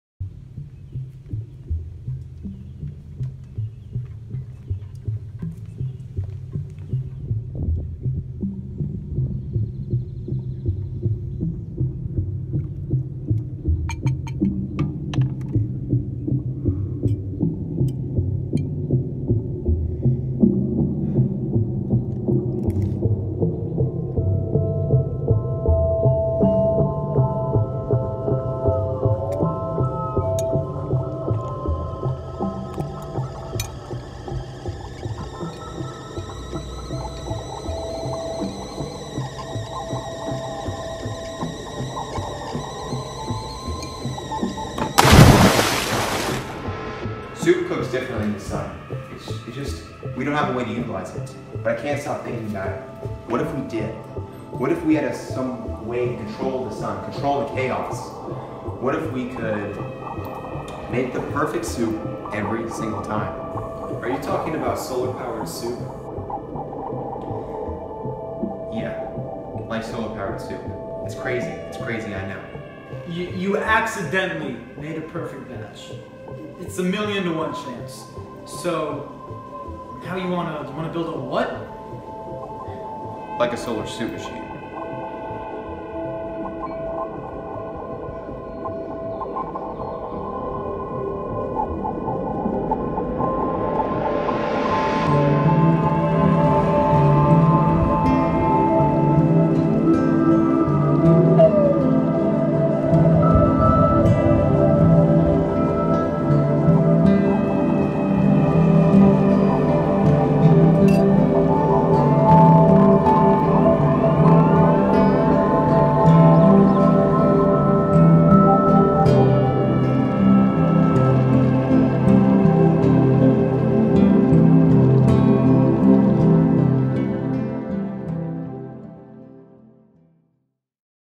Soupman's New Recipe
OG (4/20/20) (3/30/21 Remaster)

This was directly Inspired by Avengers Endgame lol.

There was one large continuity error in this video, can you find it?

This was also for a two minute short film festival.

Filmed on the Iphone 6s